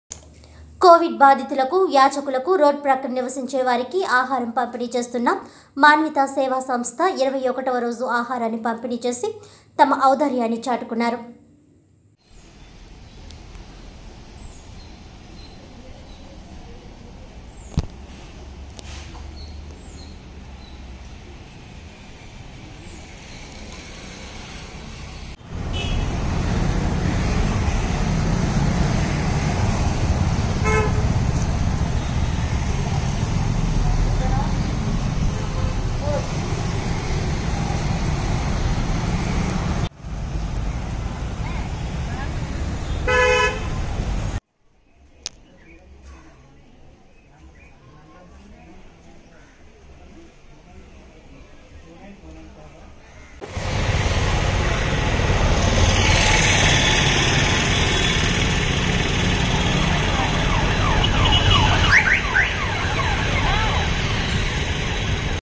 కాకినాడ- 21వ రోజు ఆహారాన్ని పంపిణీ చేసిన మాన్వితా సేవా సంస్థ _|TV7 NEWS|19-05-2021|EGDT|AP
కోవిడ్ బాధితులకు యాచకులకు, రోడ్ పక్కన నివసించే వారికి ఆహార పంపిణీ చేస్తున్న మాన్వితా సేవా సంస్థ  21వ రోజు  ఆహారాన్ని పంపిణీ చేసి తమ ఔదార్యాన్ని చాటుకున్నారు.